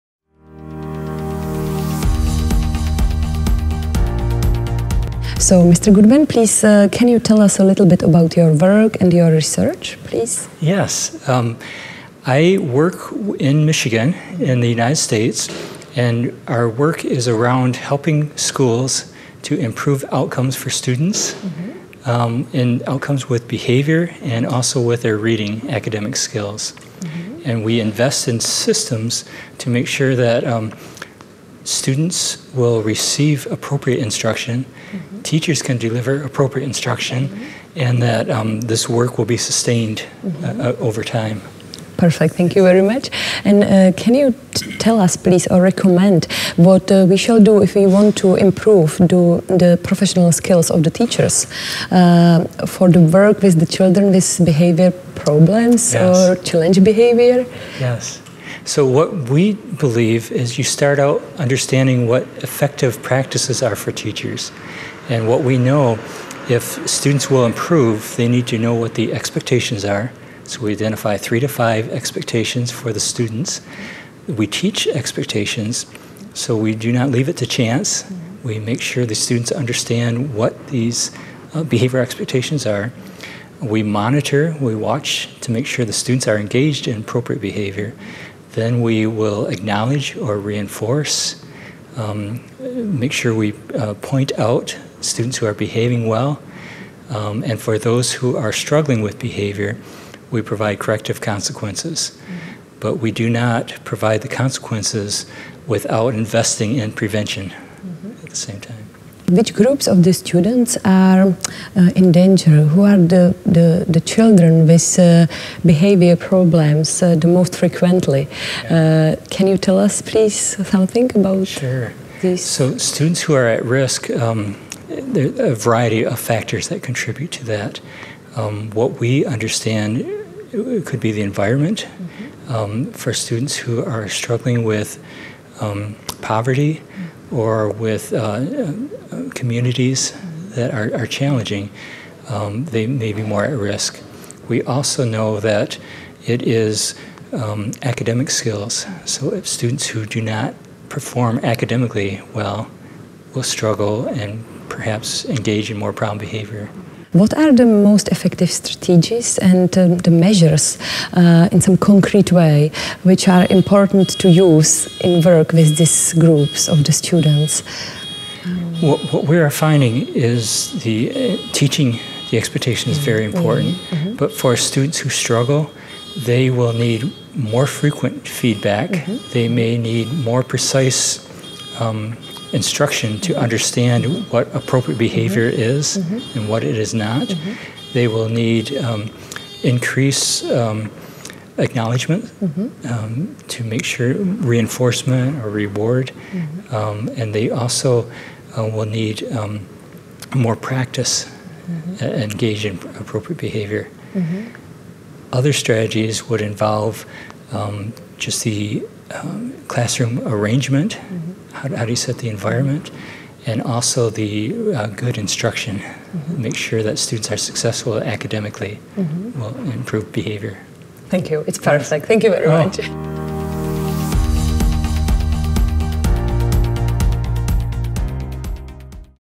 Steve Goodman
Na radost z učení a zážitek z úspěchu má právo každý.
Krátký rozhovor se Stevem Goodmanem, ředitelem Michigan’s Integrated Behavior and Learning Support Initiative a specialistou na podporu pozitivního chování žáků. Poslechněte si, jaké jsou cíle PBIS.org (Positive Behaviour Interventions and Support) a co Steve radí pro zlepšení profesionálních dovedností učitelů pracujících s dětmi s problémovým chováním.